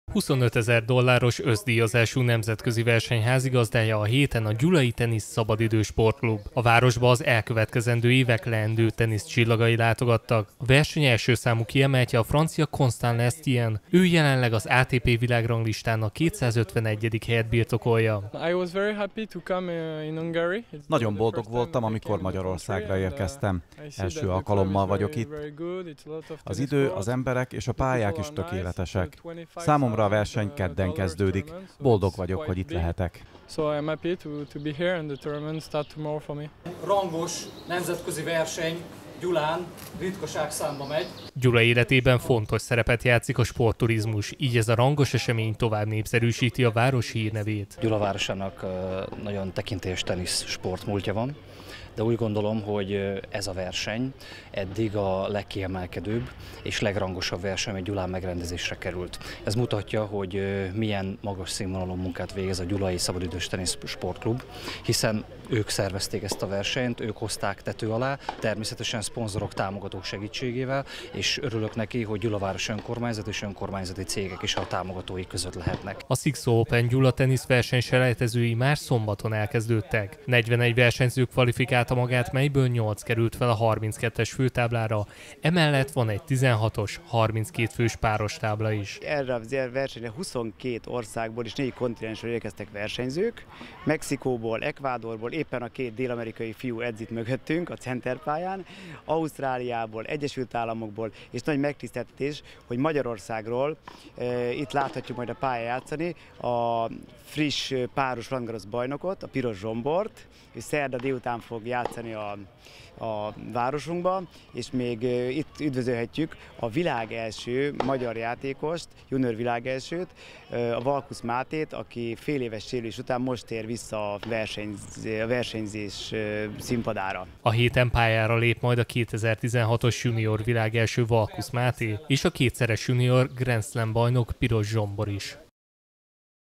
Nemzetközi teniszversenynek ad otthont Gyula
25 ezer dolláros összdíjazású nemzetközi verseny házigazdája a héten a gyulai Tenisz Szabadidő Sport Klub. A városba az elkövetkezendő évek leendő teniszcsillagai látogattak.

A verseny első számú kiemeltje a francia Constant Lestienne, ő jelenleg az ATP világranglistán a 251. helyet birtokolja.

Gyula életében fontos szerepet játszik a sportturizmus, így ez a rangos esemény tovább népszerűsíti a város hírnevét. A XIXO Open Gyula teniszverseny selejtezői már szombaton elkezdődtek. 41 versenyző kvalifikálta magát, melyből nyolc került fel a 32-es főtáblára, emellett van egy 16-os, 32 fős páros tábla is.

A héten pályára lép majd 2016-os junior világelső Valkusz Máté és kétszeres junior Grand Slam-bajnok Piros Zsombor is.